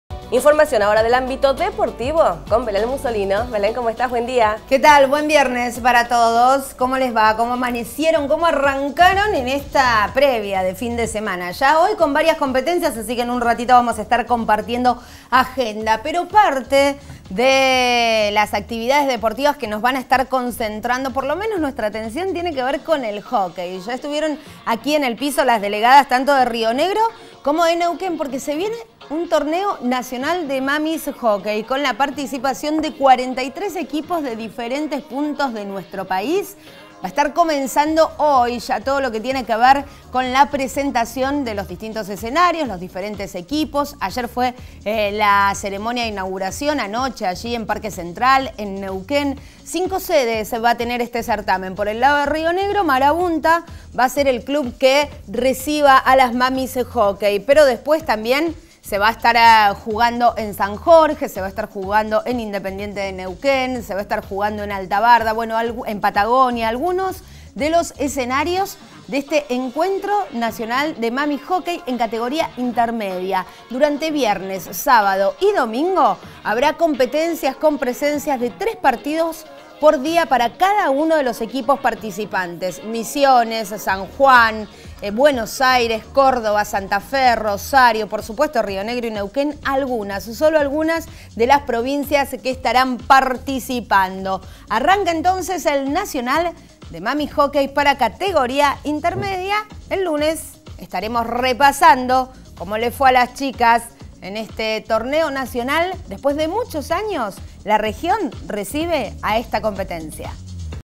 Alto Valle: Arranca el Nacional
Mamis Hockey:
El Alto Valle recibe a más de 40 equipos durante 3 días de competemcia